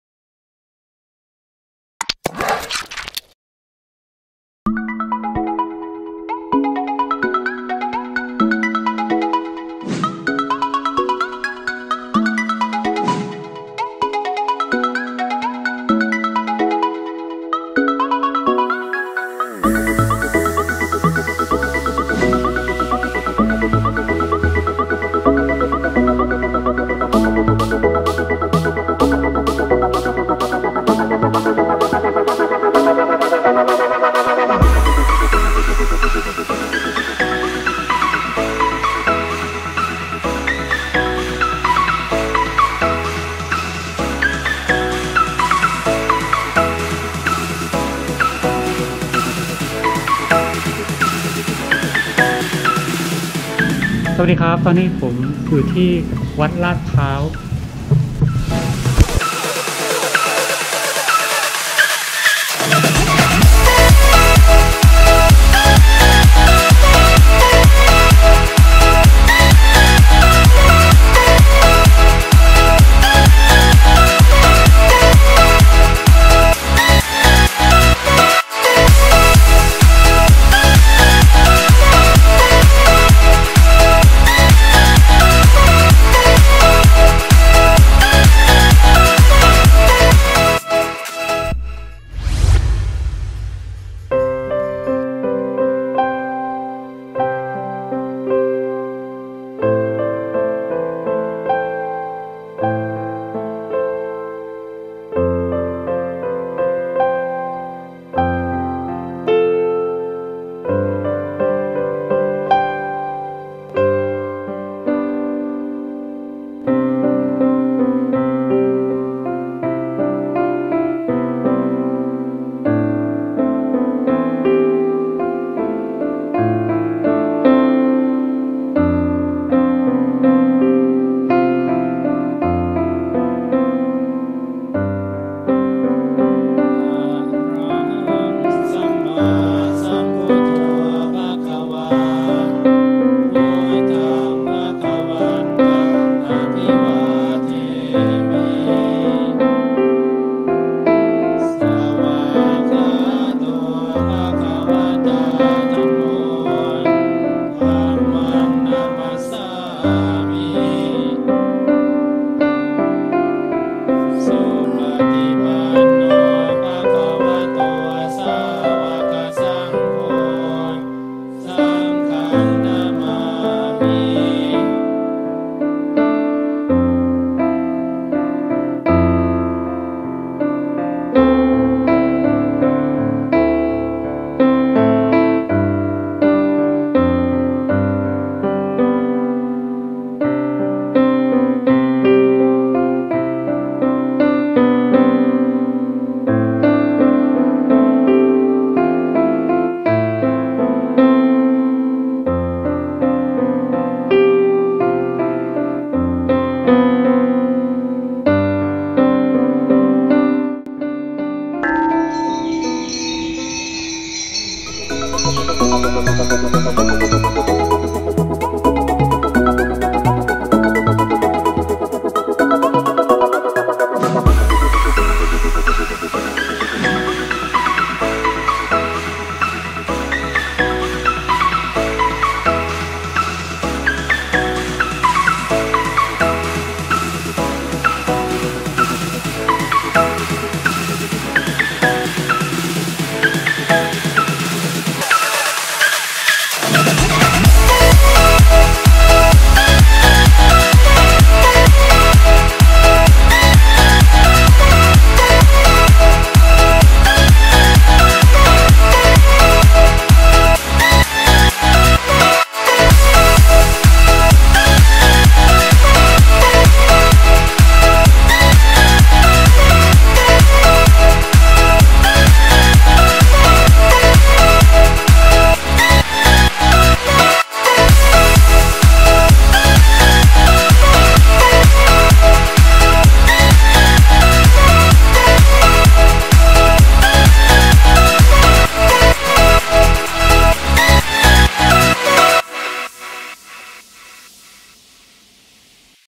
ฝเที่ยวไทย / วัดลาดพร้าว โชคชัย4 Wat Ladprao : Bangkok Thailand
วัดนี้เป็นวัดหลักสำหรับผู้คนแถวลาดพร้าว วัดอยู่ในซอยโชคชัย 4